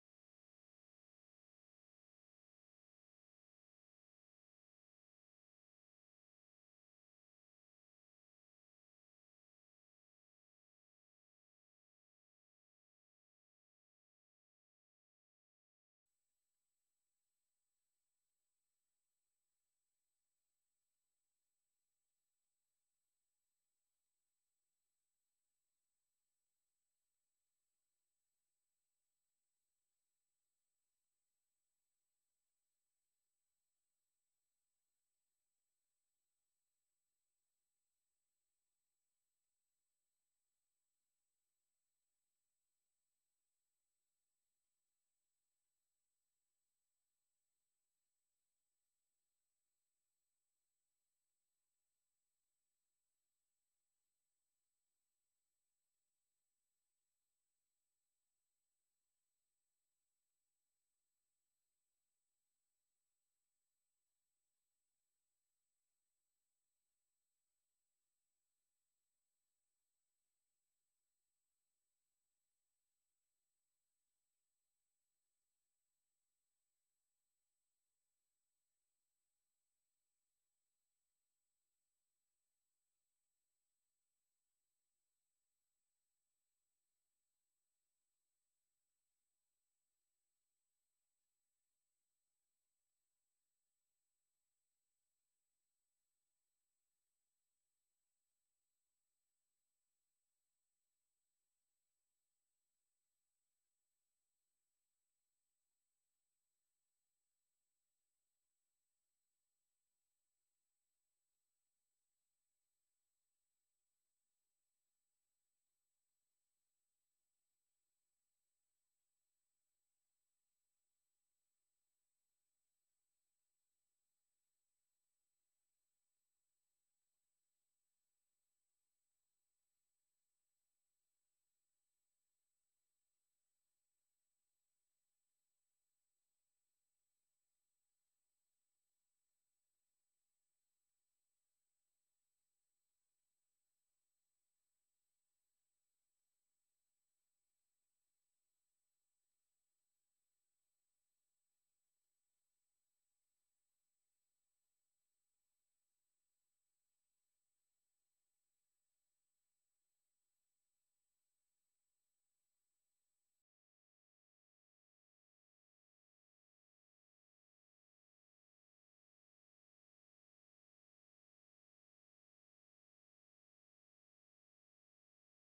Biancone (Circaetus gallicus)
Il maschio adulto giunge al nido portando un serpente predato al piccolo affinchè se ne alimenti.